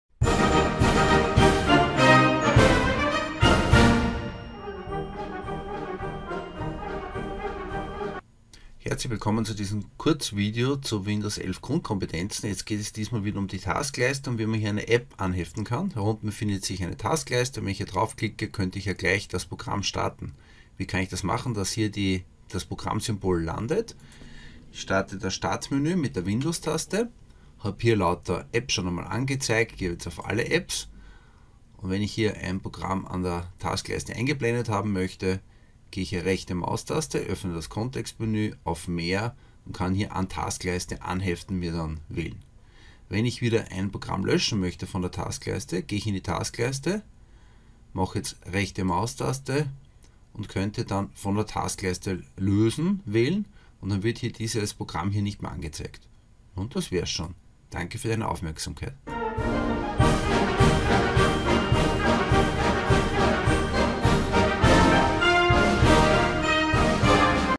38 Windows 11 - An der Taskleiste eine App anheften und wieder entfernen
Ich zeige hier, wo man an der Taskleiste eine App anheften und wieder entfernen kann. Dieses Video ist Teil meiner Playlist Windows 11 Grundkompetenzen / Grundlagen, die sich an Einsteiger*innen in das Betriebssystem richtet.